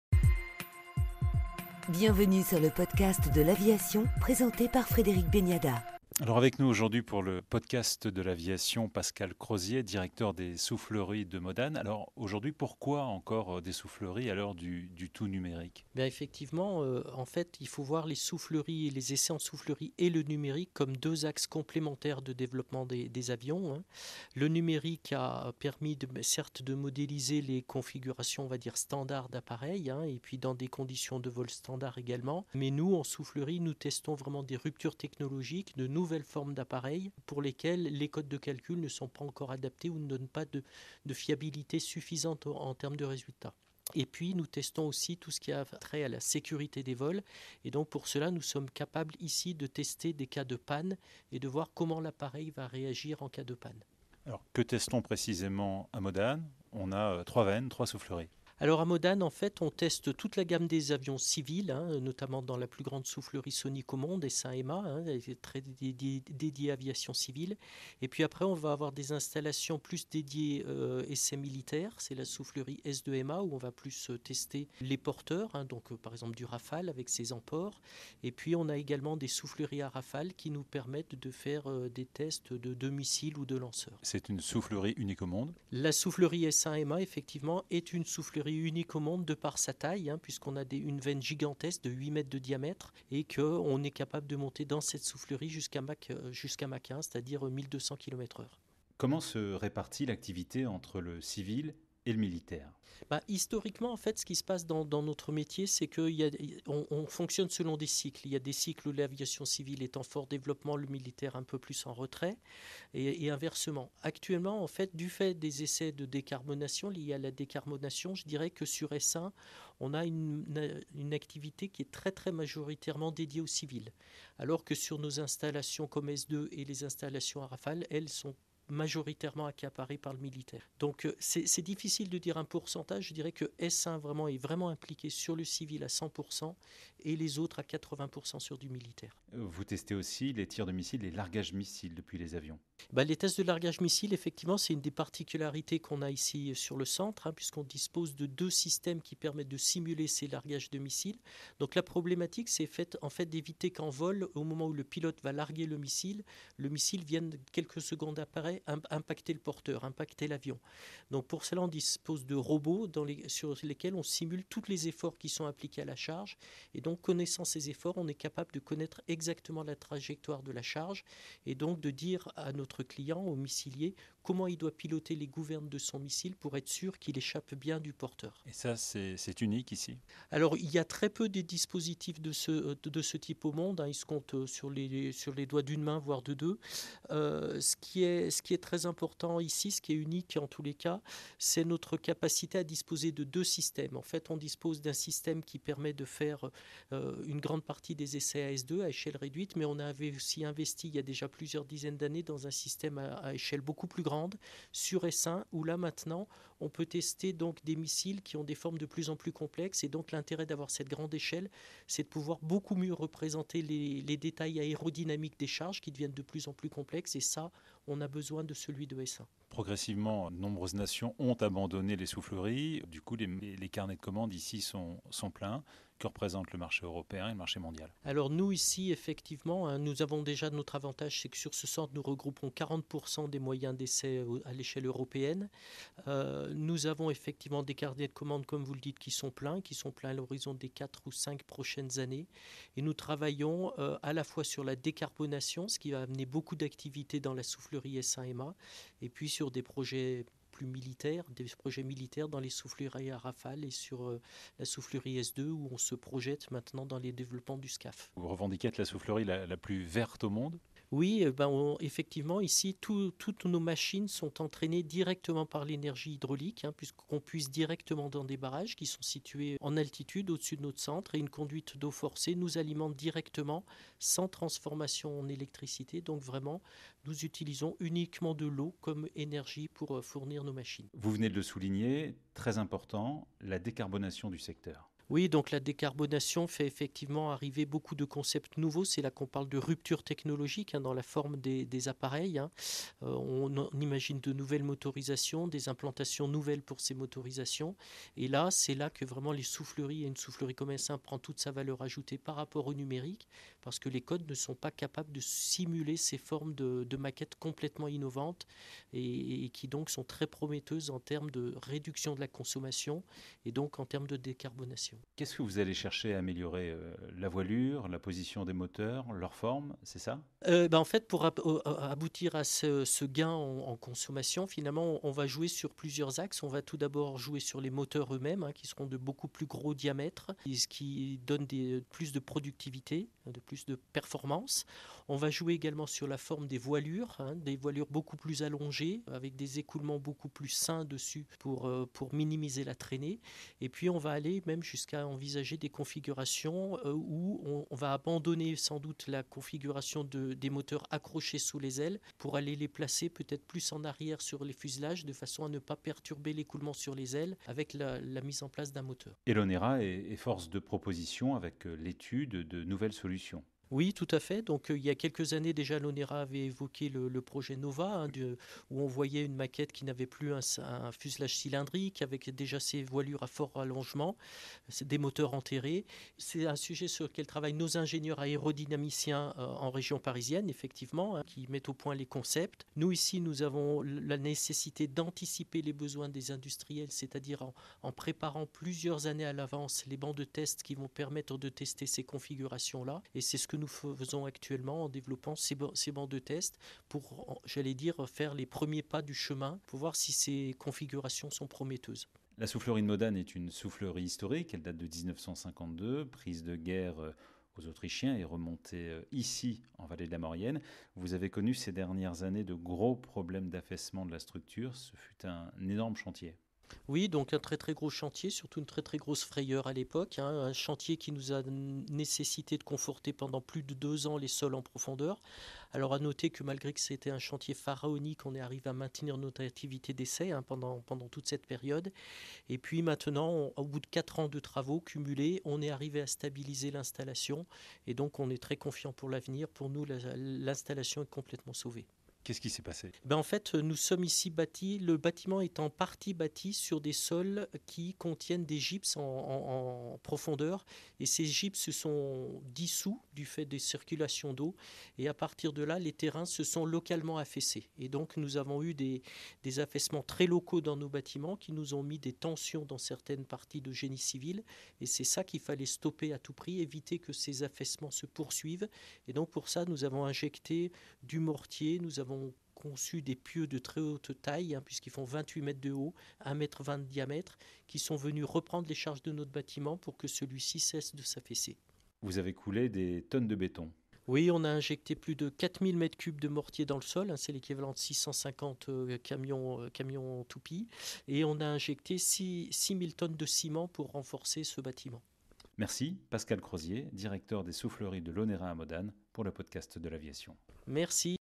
Entretien avec Pascal Crozier, directeur des souffleries de l'ONERA
Après avoir exposé l'avantage et la complémentarité des souffleries physiques par rapport aux simulations numériques, Pascal crozier décrit les missions des souffleries de Modane, qui sont aussi bien orientées vers les essais pour l'aviation commerciale que militaire et les essais de tirs de missiles. Uniques par leur taille et leurs capacités, ces souffleries ont des carnets de commandes constamment pleins. Les travaux actuels sont principalement axés sur la décarbonation de l'aviation (tests des nouveaux concepts) et les travaux préparatoires pour le SCAF (Système de combat aérien du futur).

Il évoque enfin les grandes inquiétudes qui pesaient ces dernières années sur la soufflerie, victime de l'affaissement de sa structure, et son sauvetage.